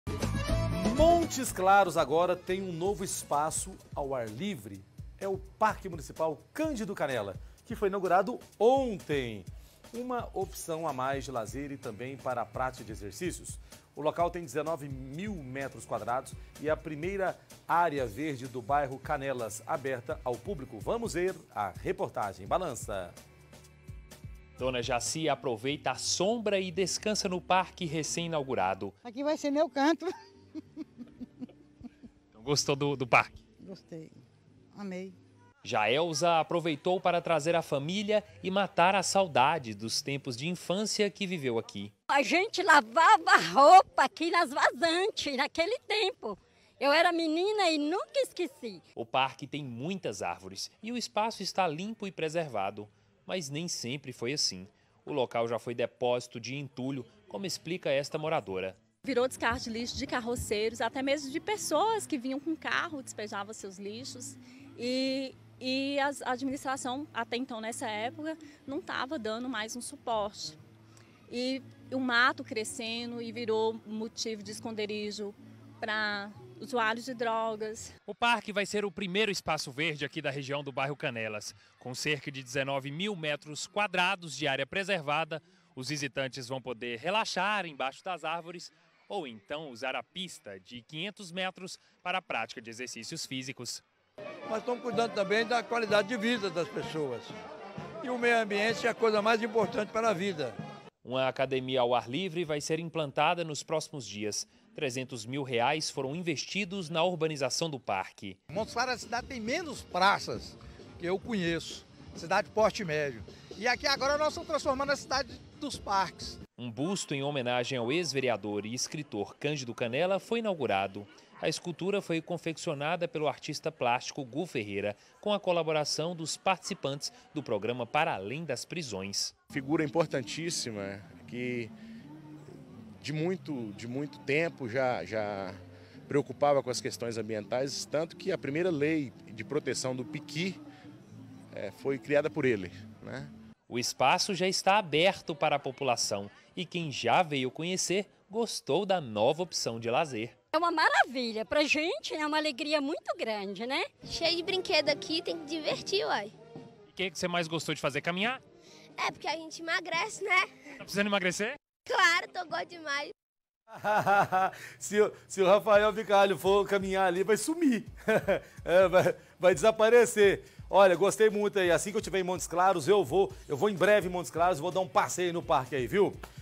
Parque Municipal é inaugurado em Montes Claros
Montes Claros, agora, tem um um novo espaço ao ar livre. É o Parque Municipal Cândido Canela, que foi inaugurado, ontem (5). Uma opção a mais de lazer e também para a prática de exercícios. O local tem 19 mil  m² e é a primeira área verde do Bairro Canelas aberta ao público.